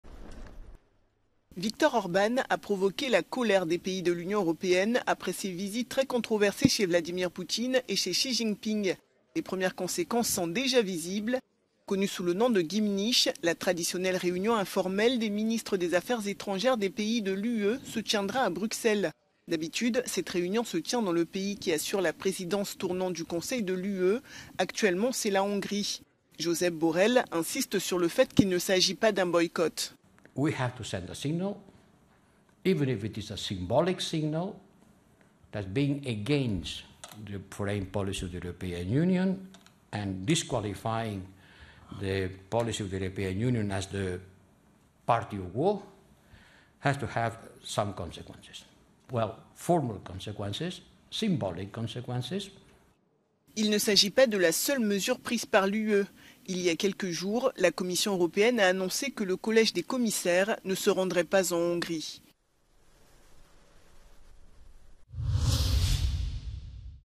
Josep Borrell envoie "un signal" à la Hongrie de Viktor Orbán
Josep Borrell snobe Budapest après les visites de Victor Orban en Chine et en Russie. Le chef de la diplomatie européenne veut lancer un signal symbolique à la Hongrie. La réunion informelle des ministres des affaires étrangères prévue à Budapest à la fin du mois d'août se tiendra à Bruxelles.